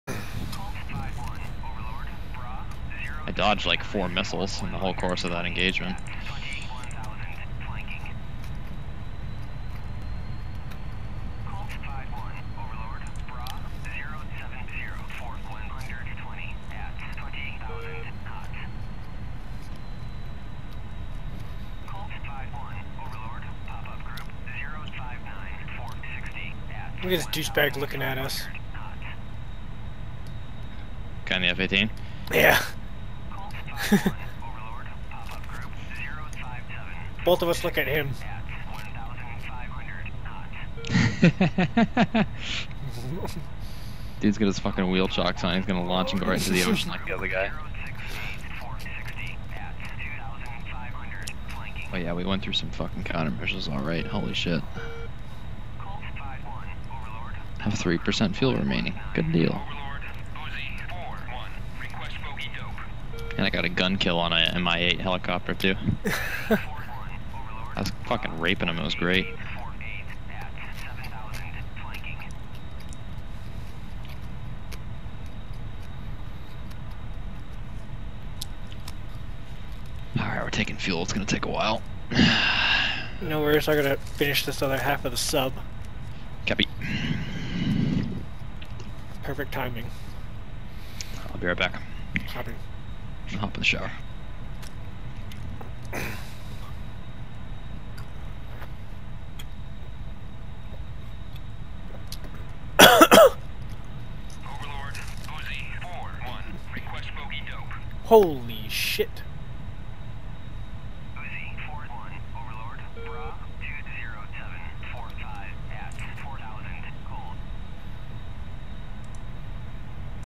Refuel break on the Carrier (Snack Break)
Just minding our own business on the Carrier Deck, and BOOM. watch what happens all the way to the end!! Sorry about the chewing, was trying to scarf down a B.M.T. Sub while refueling.